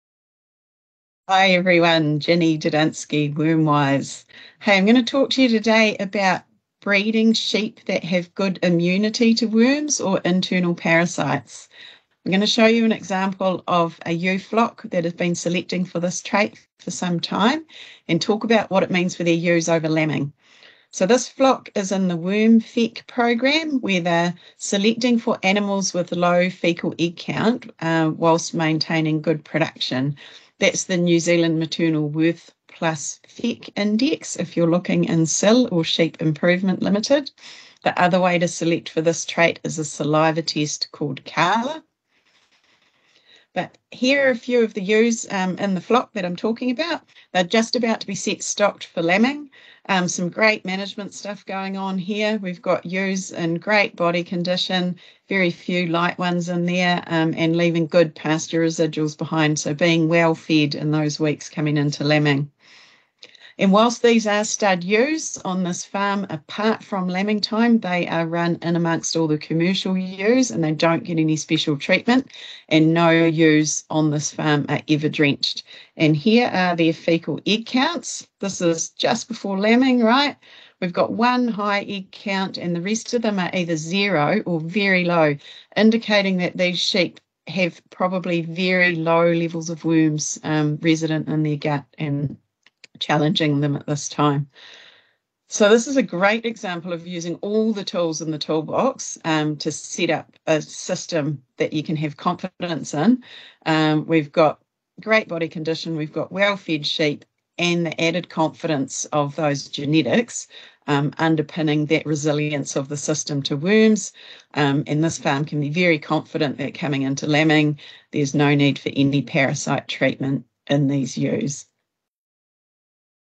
Benefits of worm-resistant sheep at lambing time
Wormwise programme manager Ginny Dodunski shows an example of ewes from a worm-resistant flock just prior to lambing.